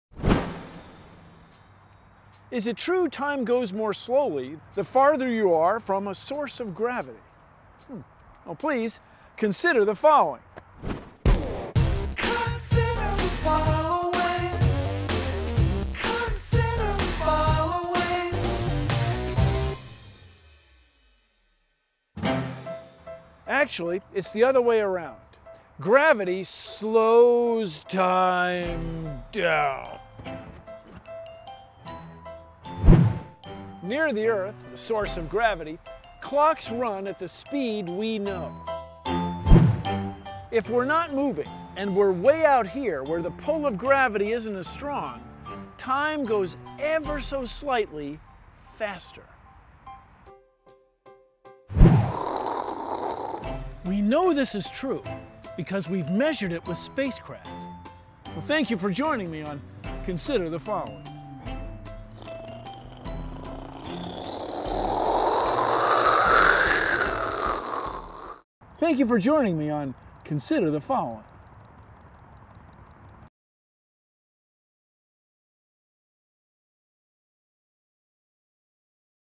Bill Nye Consider the Following: Does Time Go Faster On Earth Than In Space?
Bill Nye answers the question "Does Time Go Faster On Earth Than In Space" in this Toshiba Vision Consider the Following series video. for all Bill Nye videos answering questions submitted by Facebook users.